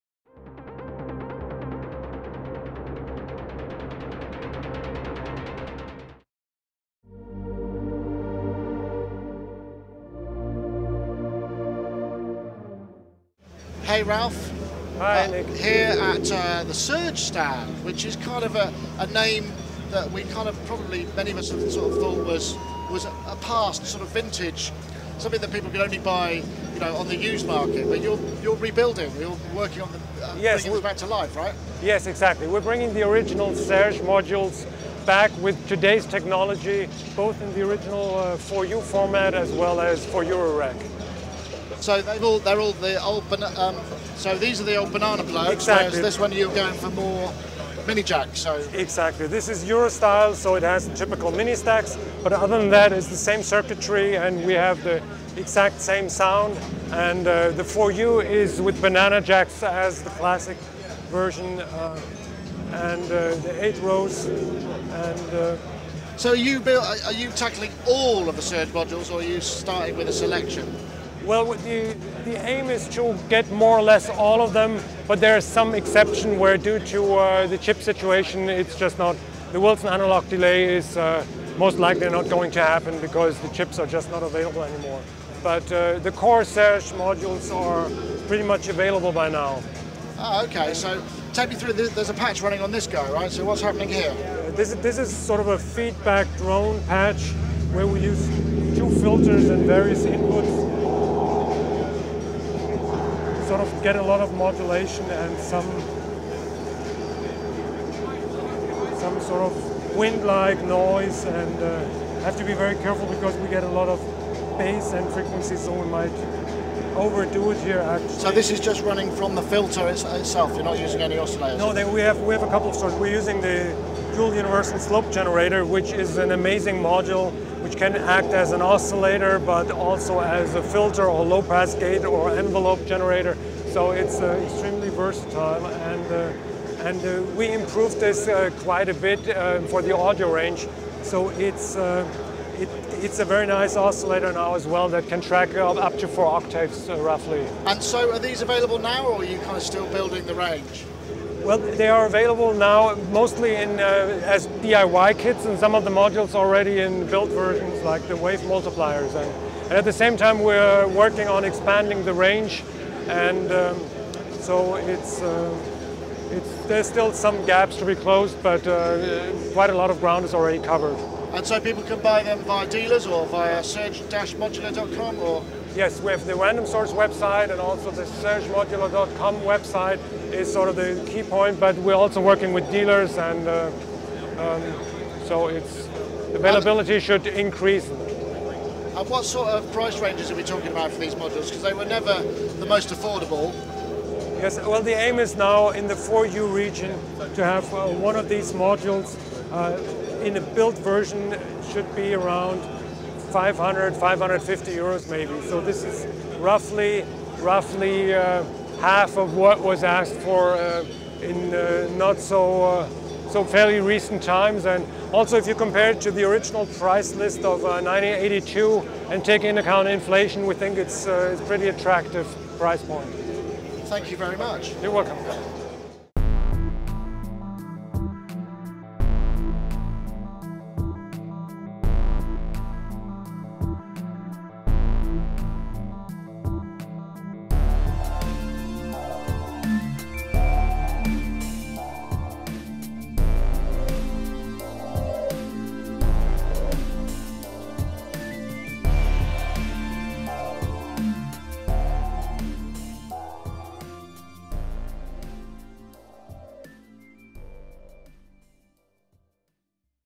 Superbooth 2017: Serge Are back
Reissues of the Serge modules in 4U and Eurorack coming soon